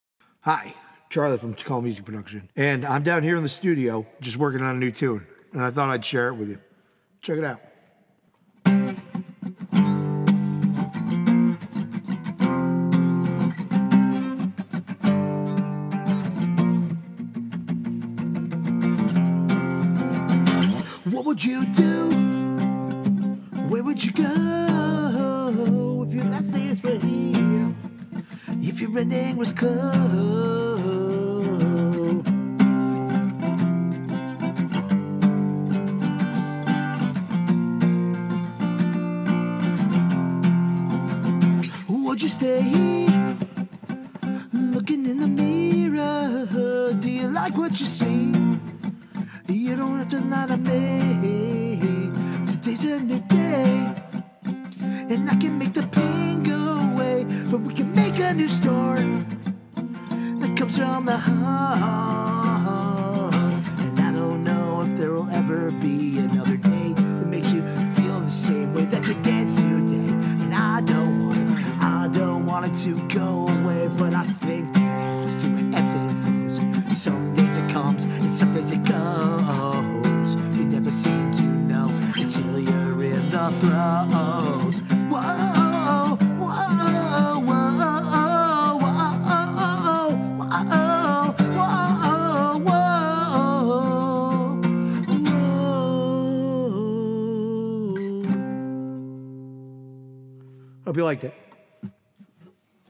Wrote A New Song!!! - IT JUST POPPED INTO MY HEAD 🤯
I was driving to work when inspiration hit me, I recorded myself singing the only line I had come up with and organically just came out with the rest in one take! Where the h*ll did it come from? Spilt Milk is already working on the concept and putting it together!